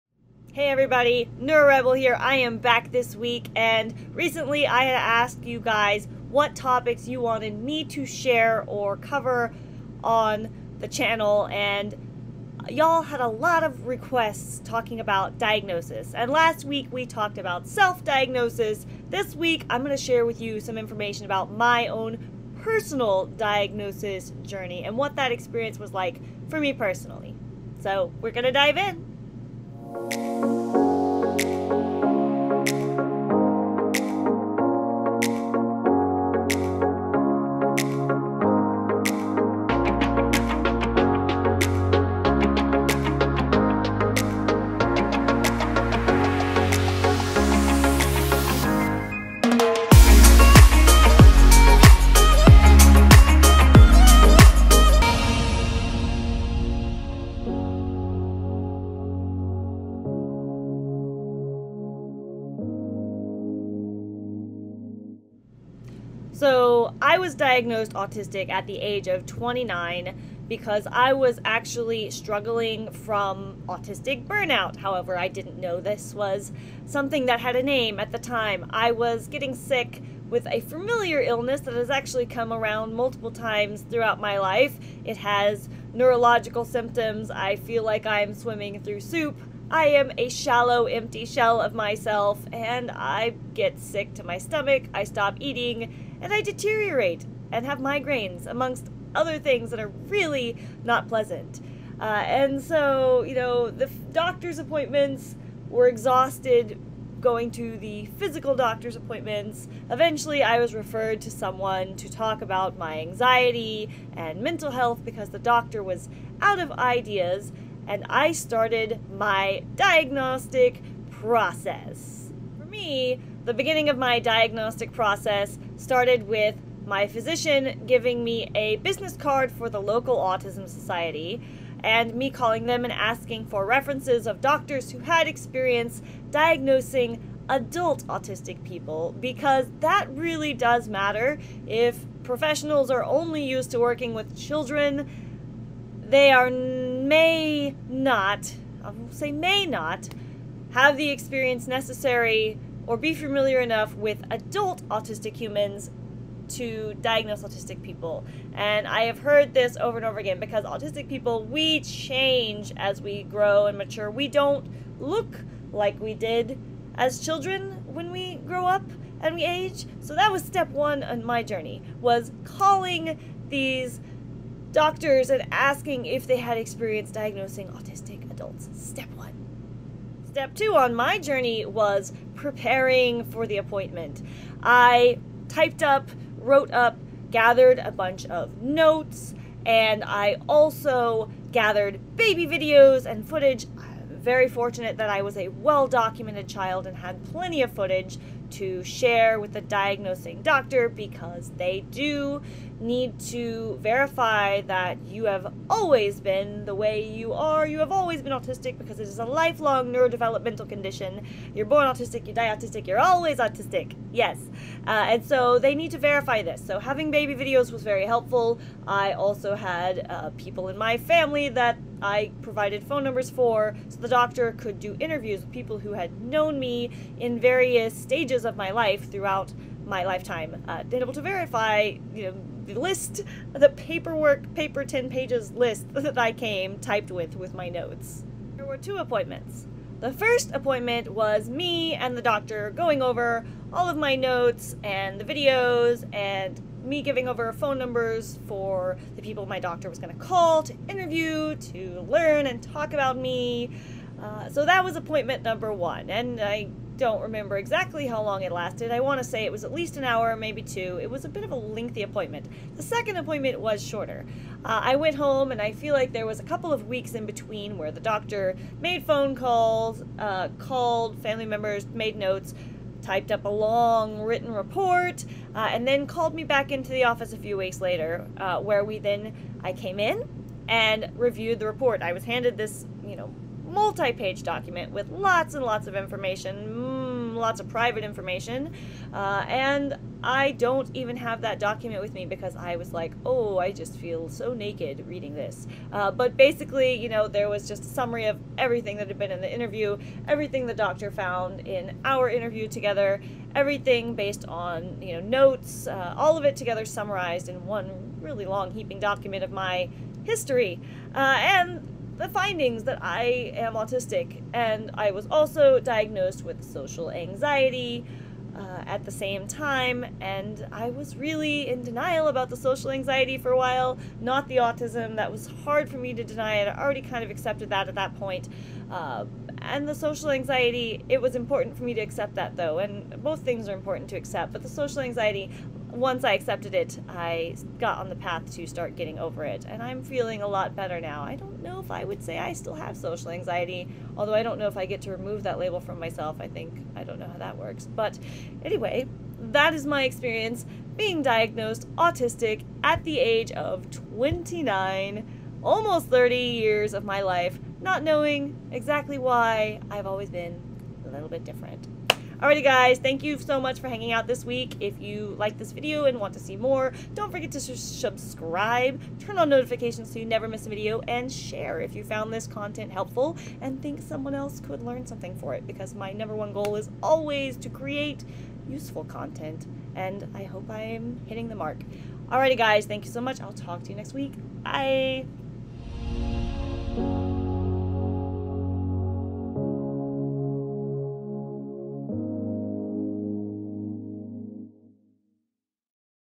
My Late Autism Diagnosis Journey
Hey everybody, NeuroRebel here I am back this week. And recently I asked you guys what topics you wanted me to share or cover on the channel. And y'all had a lot of requests talking about diagnosis. And last week we talked about self-diagnosis this week, I'm going to share with you some information about my own personal diagnosis journey and what that experience was like for me personally.